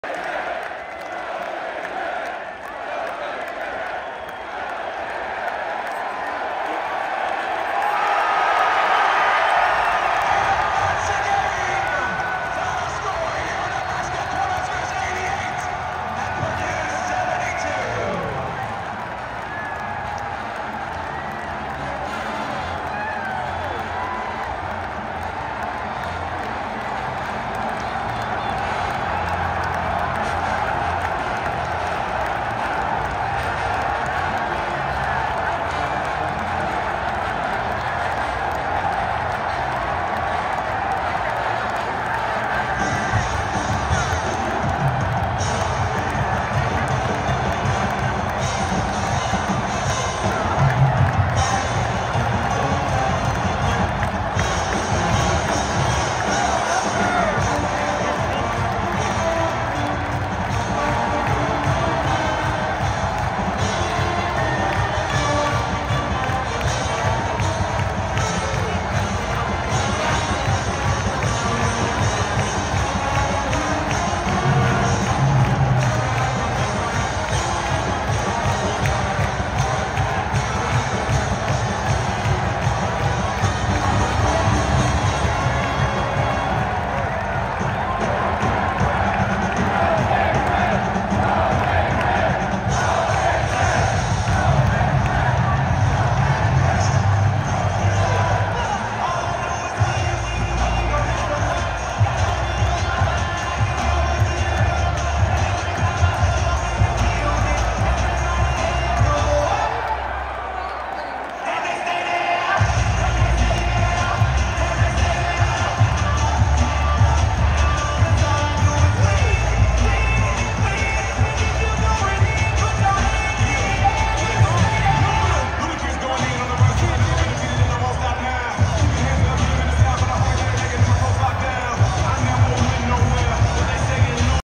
Husker fans storm the court after Nebraska basketball’s 88-72 win over No. 1 ranked Purdue
Nebraska fans stormed the court at Pinnacle Bank Arena as the Huskers won 88-72 over No. 1 ranked Purdue. It's NU's first win over a No. 1 ranked team since 1982.